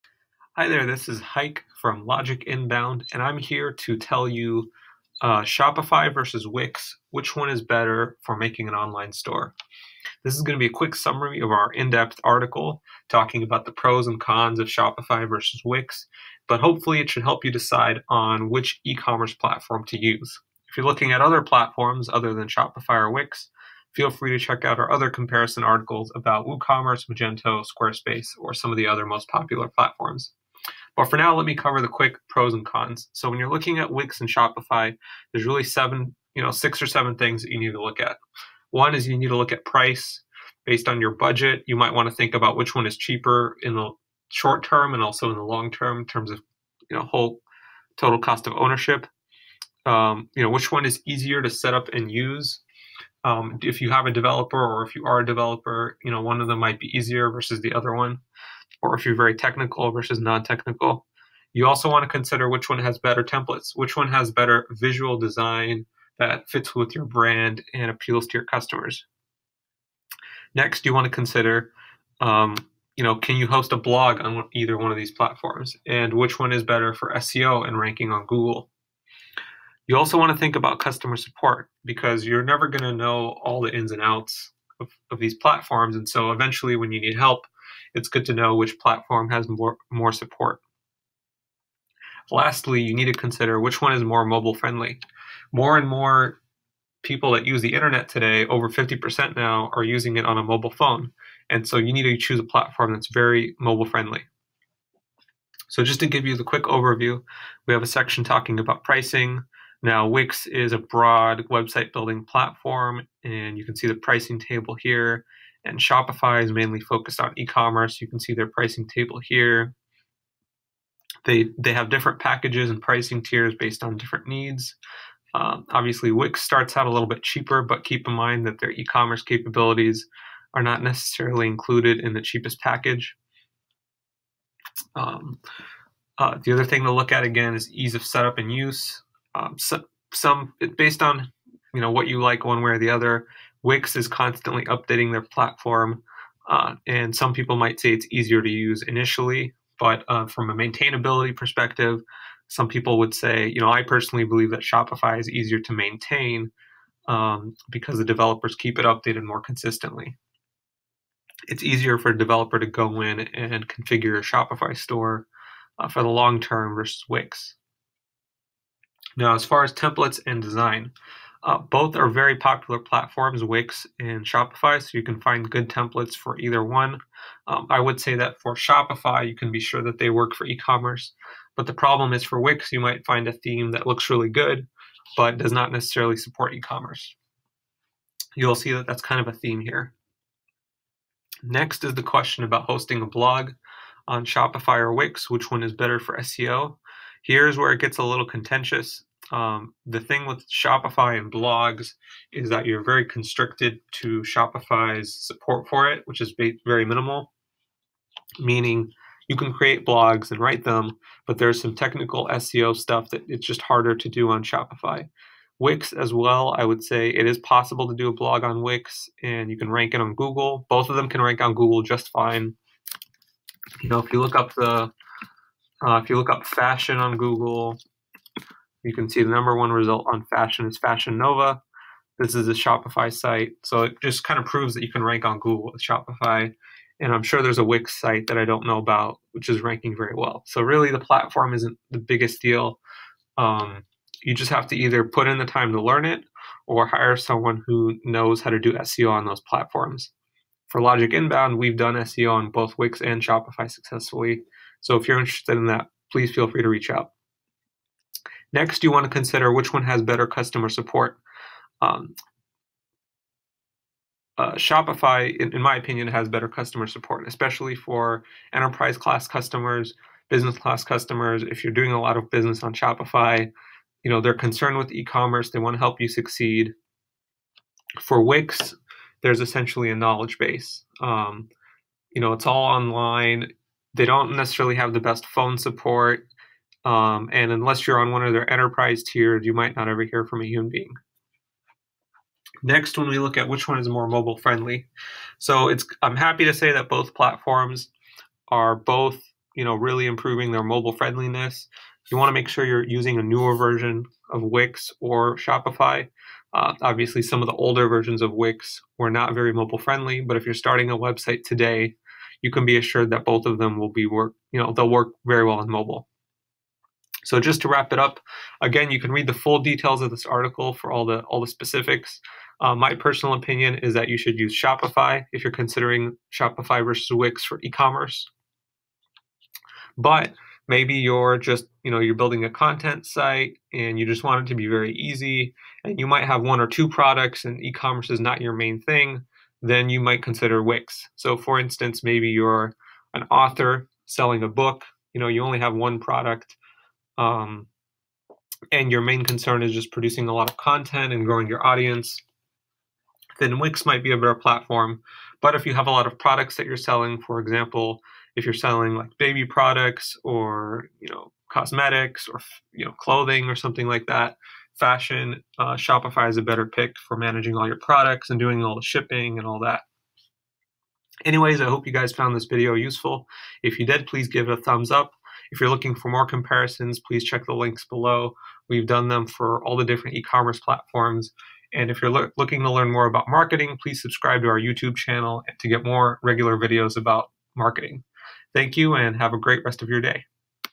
Shopify vs Wix - For an eCommerce Store Website Which One is Better in 2018?
Shopify or Wix? Which platform should you choose to make your ecommerce website?

Read the full article to learn all the details about the pros and cons of Shopify and Wix: